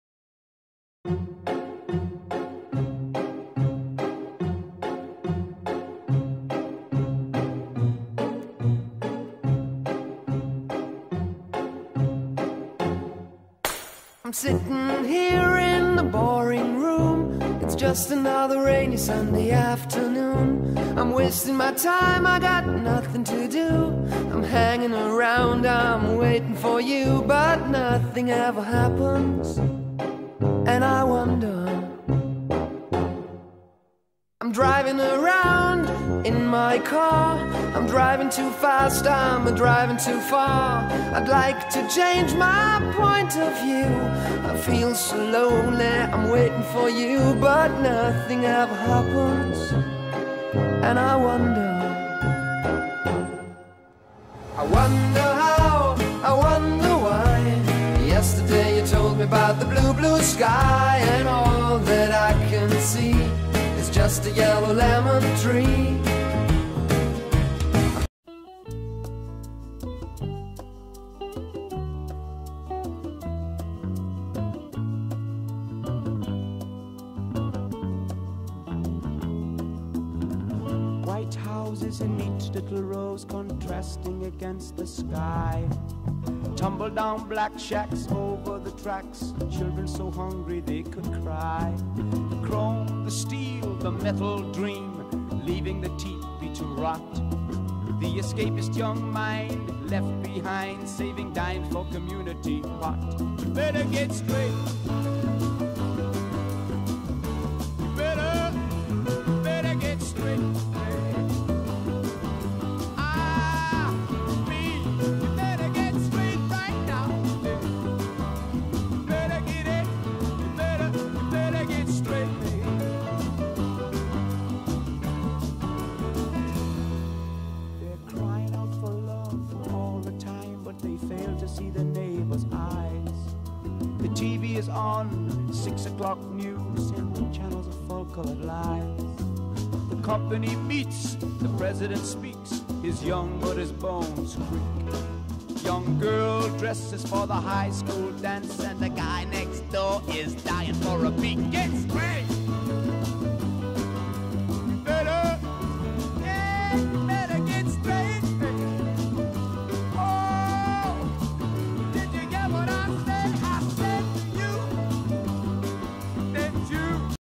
Mono lento
Película realizada con el programa Movie Maker sobre las personalidades animales :)